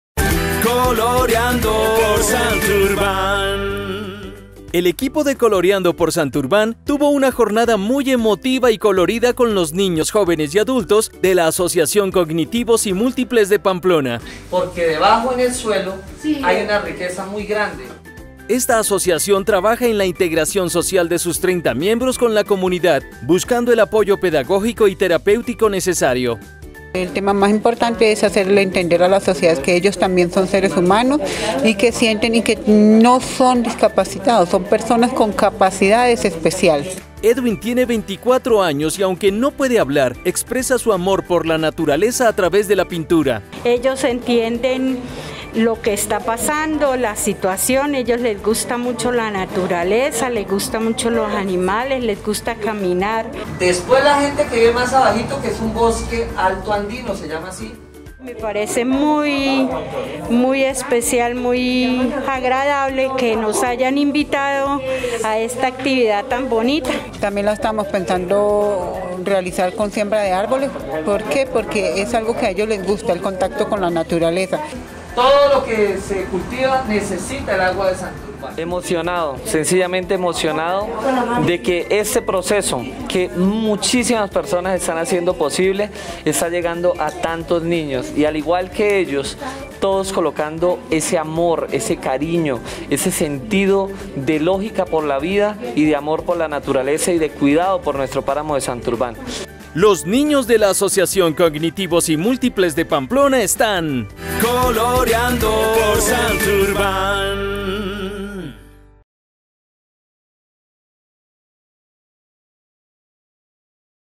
RECORRIENDO NUESTRO TERRITORIO - ASOCIACIÓN COGNITIVOS Y MÚLTIPLES DE PAMPLONA
Compartimos una jornada de pintura, con unas personitas muy especiales; que nos demostraron que hay otras formas distintas a la voz, para expresar el amor por nuestra naturaleza,  La asociación de cognitivos y múltiples de Pamplona.